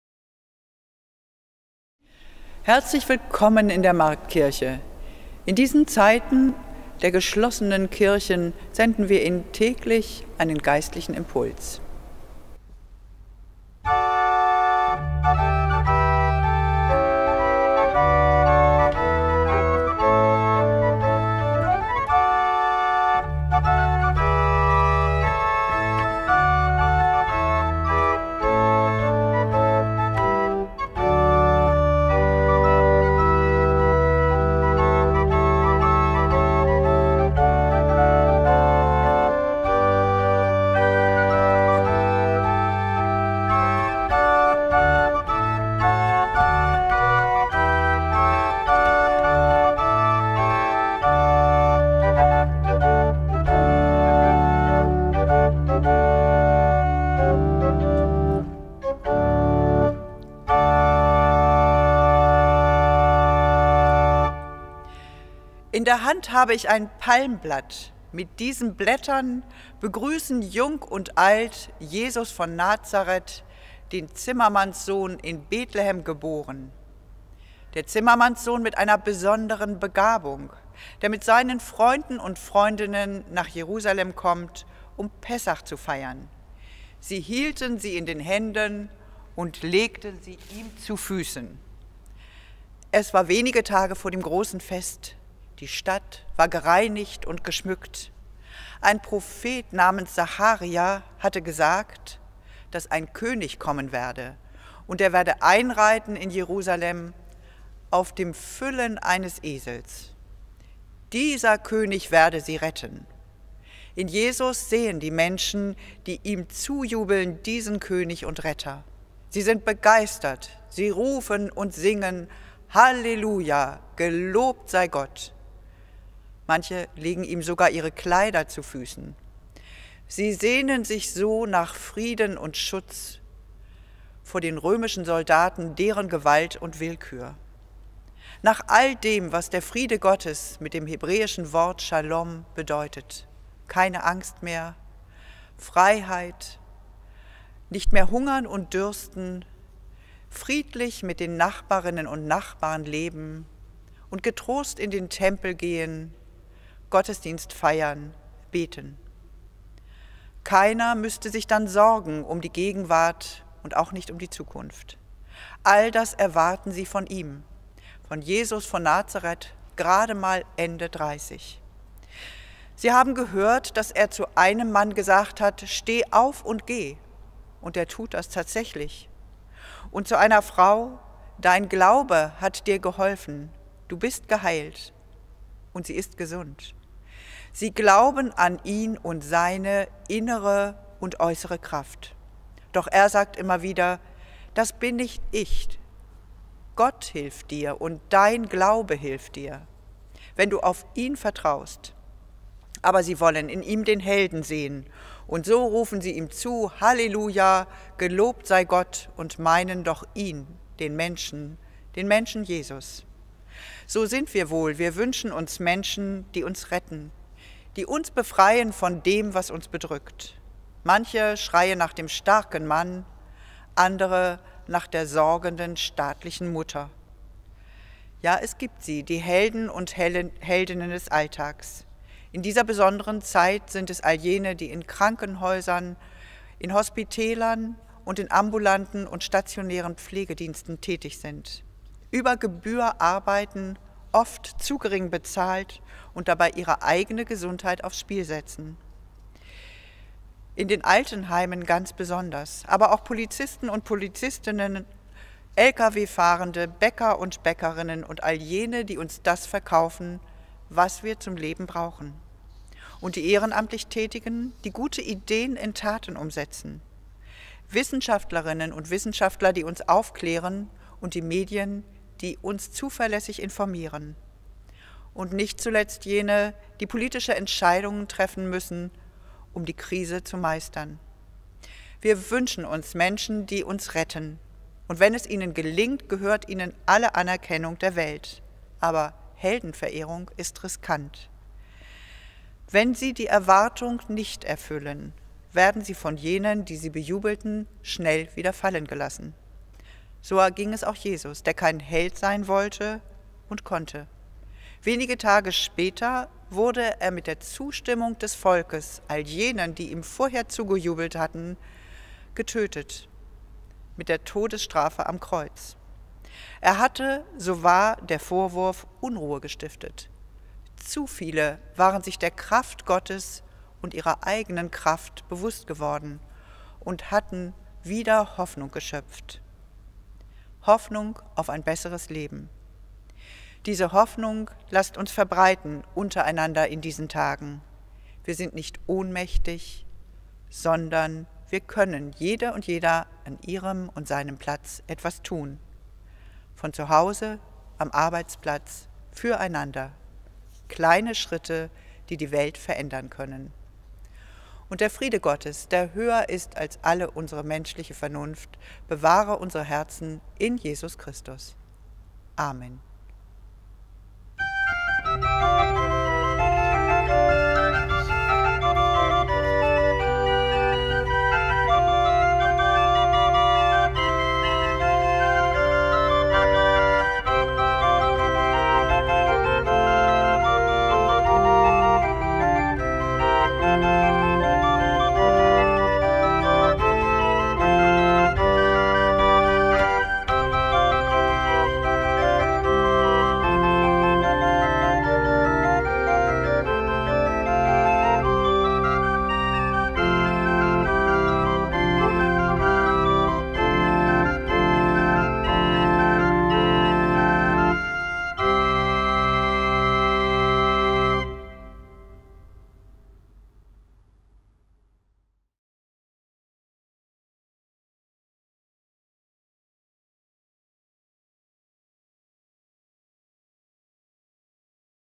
Geistlicher Impuls am 05.04.2020
In Zeiten geschlossener Marktkirche grüßen wir Sie täglich mit einem 'Geistlichen Impuls' aus der Marktkirche Hannover.